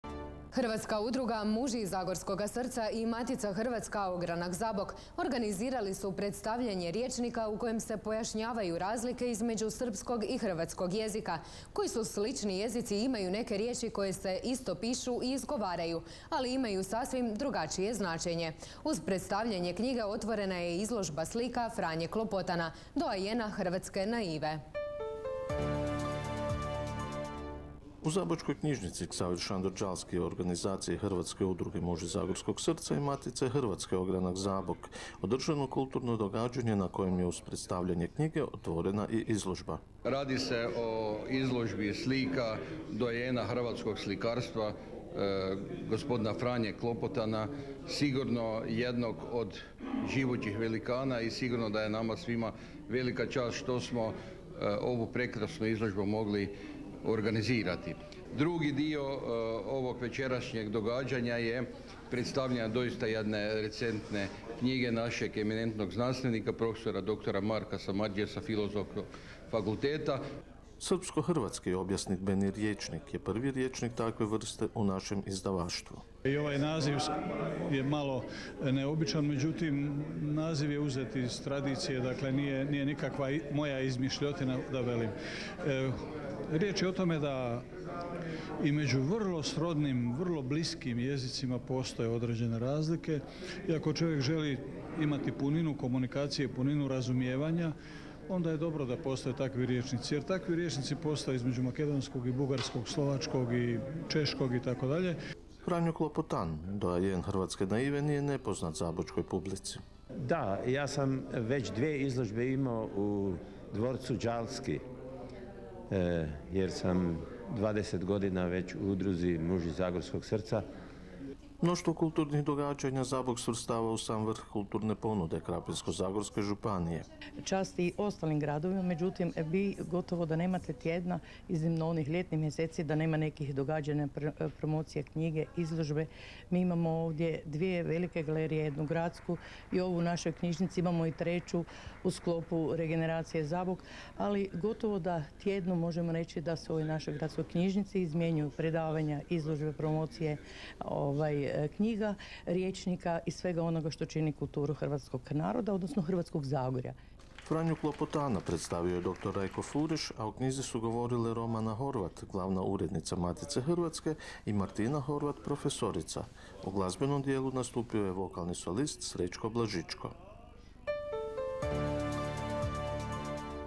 Franjo Klopotan - velikan naivnog slikarstva
Izložba u galeriji Ksavera Šandora Gjalskog , Zabok 2016.